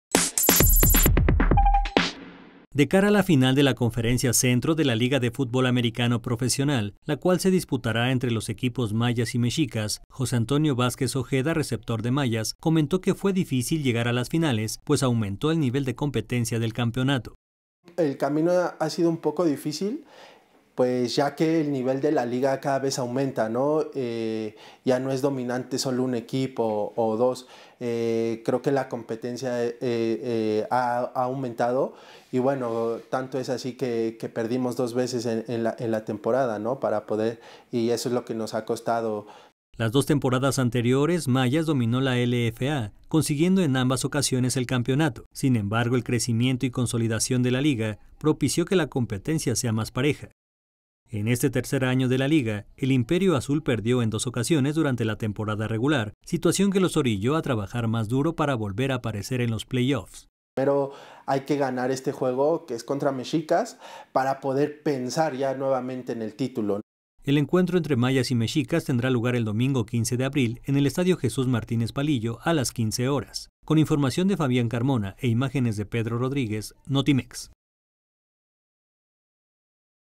Complicado camino de Mayas llegar a finales de LFA
De cara a la final de la Conferencia Centro de la Liga de Futbol Americano Profesional, la cual se disputará entre los equipos “Mayas” y “Mexicas”, José Antonio Vázquez Ojeda receptor de Mayas, comentó que fue difícil llegar a las finales pues aumentó el nivel de competencia del campeonato.